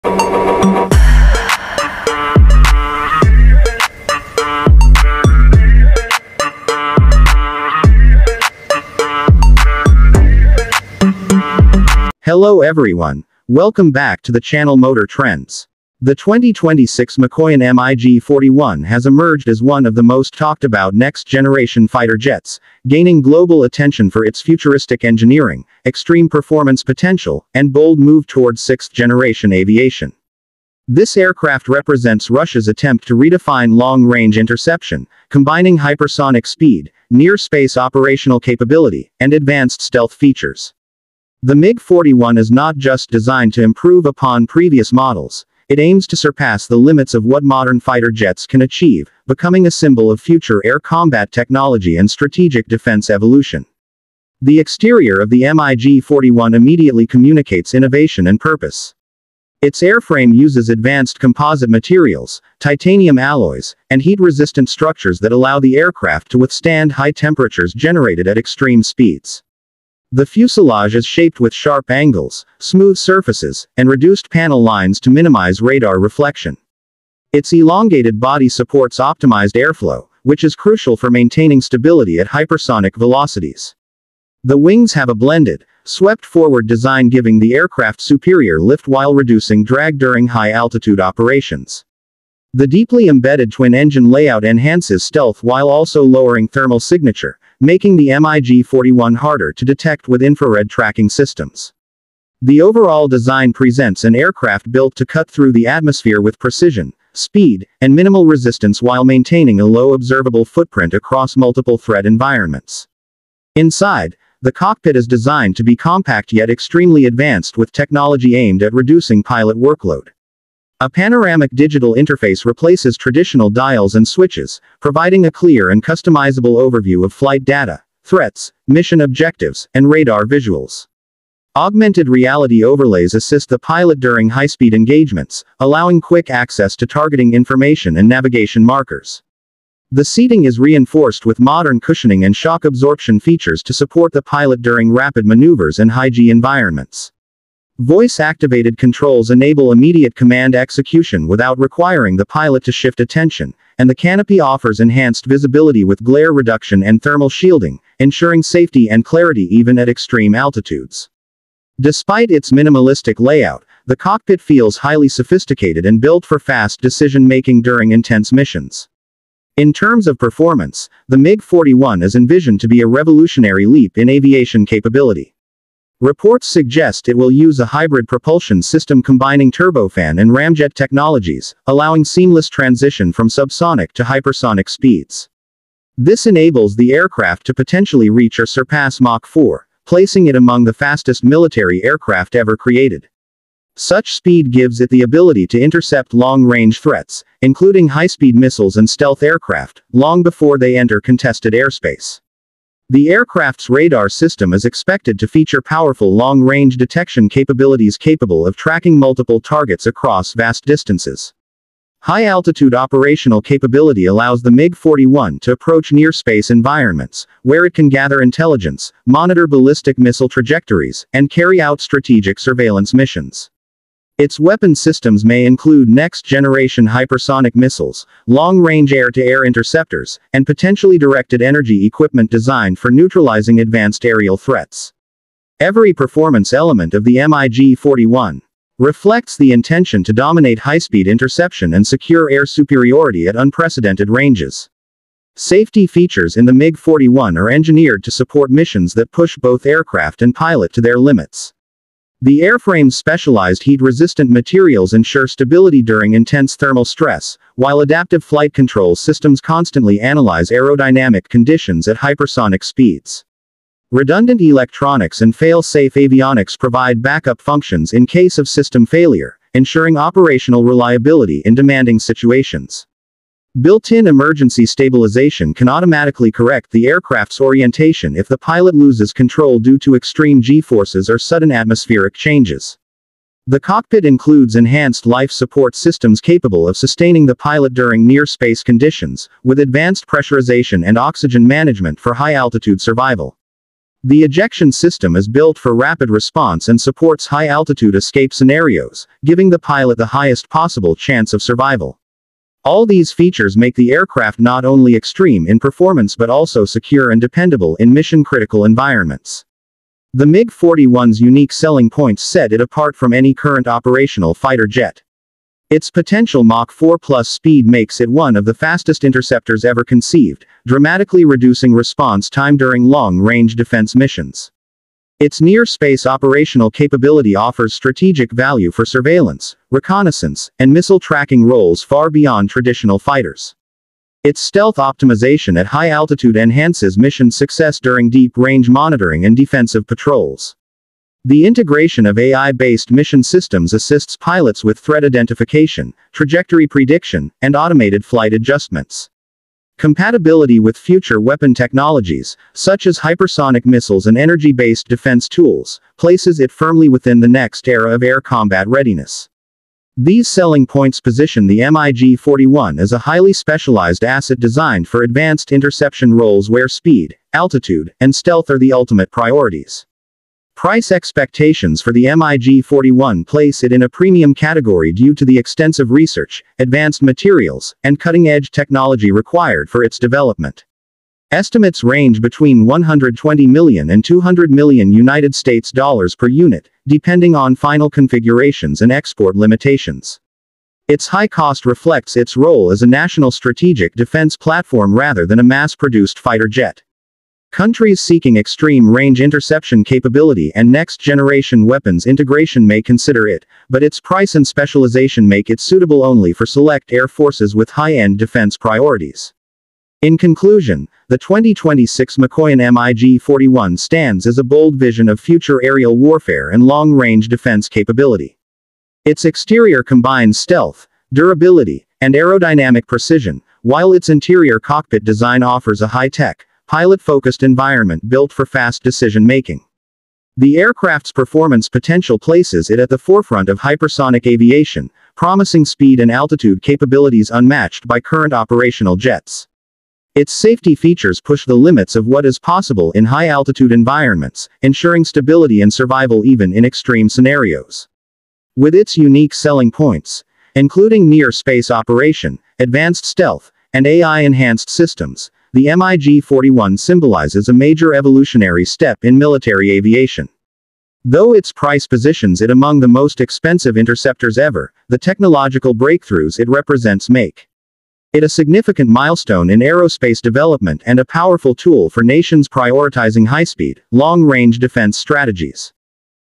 MiG-41 Explained – Russia’s Sixth-Generation Fighter Jet
The 2026 Mikoyan MiG-41 is one of the most advanced next-generation fighter jets ever developed, designed for hypersonic speed, near-space operations, and unmatched long-range interception capability. This video explores its exterior design, interior technology, performance features, safety systems, unique selling points, expected price, and why it stands out as a potential game-changer in future air combat. From its futuristic materials to its AI-powered avionics and extreme altitude capability, the MiG-41 represents a bold step into sixth-generation aviation. Watch the full review to understand what makes this aircraft one of the most anticipated defense projects in modern aerospace.